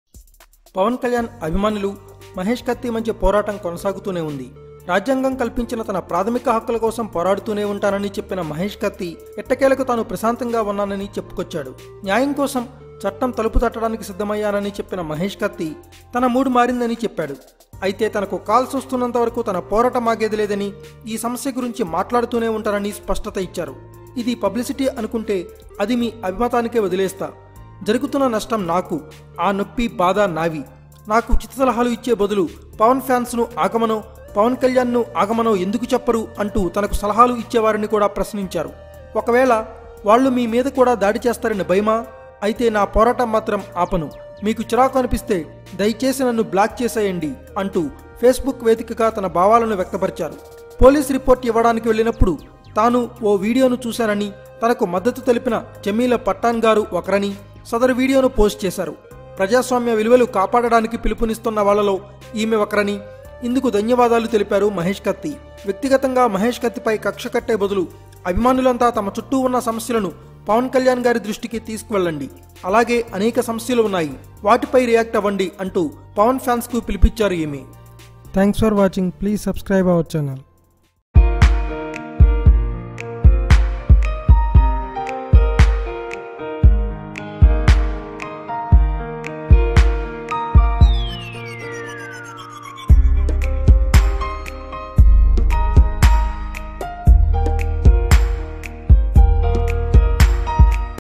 Kathi Mahesh Again Controversial Comments On Pavan Kalyan | Mahesh Kathi Vs Pawan Kalyan Fans
Watch & Enjoy Kathi Mahesh Again Controversial Comments On Pavan Kalyan | Mahesh Kathi Vs Pawan Kalyan Fans